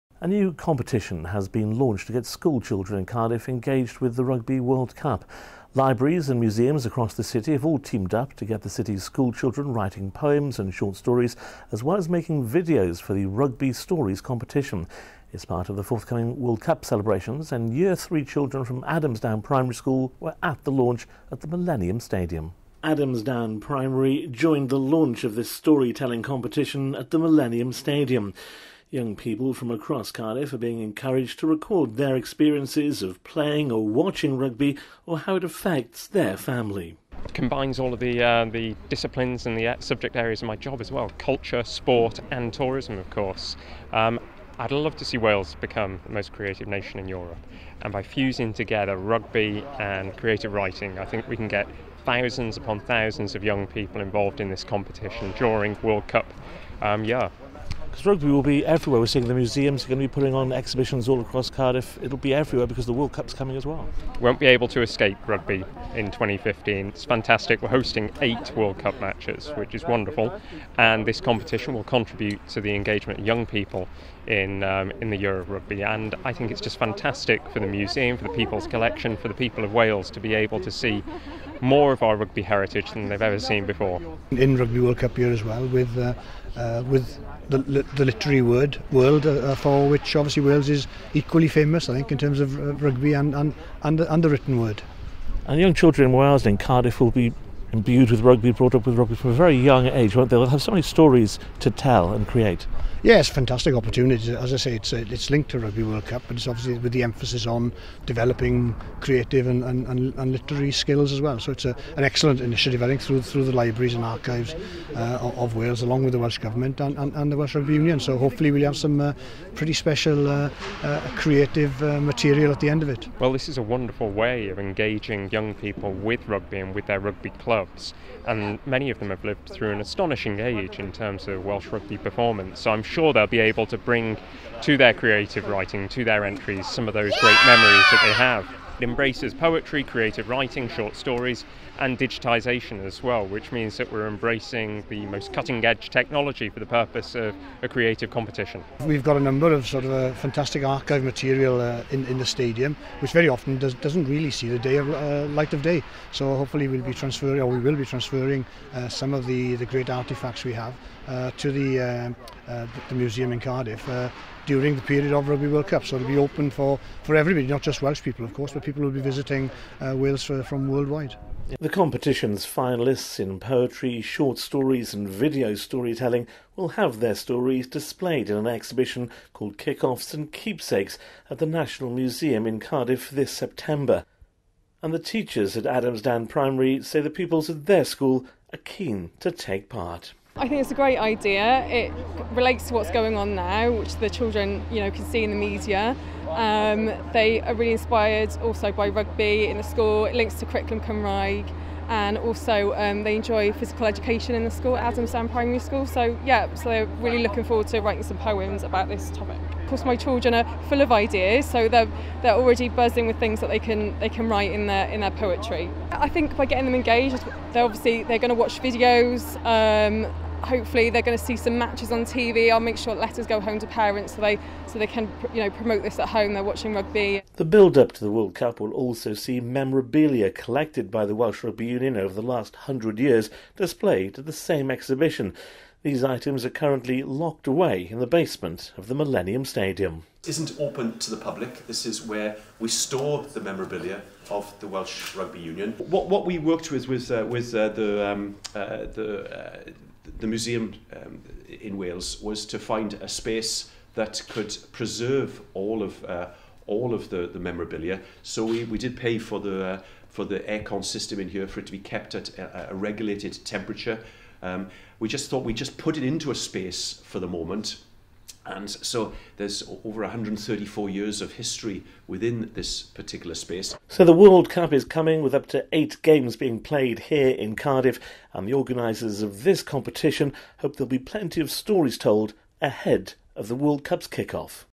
WRU STORIES FINAL
WRU launches a Story telling competition for school children across Cardiff.  They can tell their rugby experiences in poetry, short stories or video.